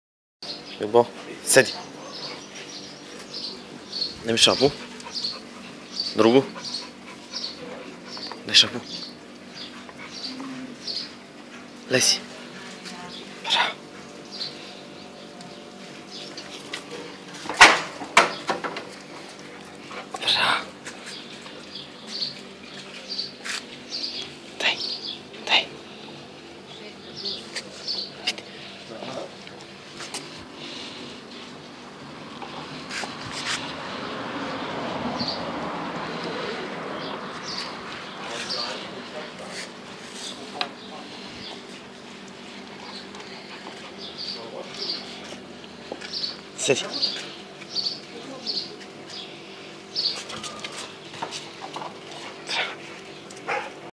Funny Dog Playing (Staffordshire Terrier)
Playing with my dog in my backyard...He is hilarious, 3.5 years old Staffordshire terrier.  This video is set up as proof that Staffordshire terriers can be so gentle dogs, and not only aggressive dogs. Hate stereotypes...Every dog is different, and it's all in education.
Spend time with your dog , take them for a walk, get to know them well with other dogs . We all love attention , so they did too. Wish u great day with your dogs...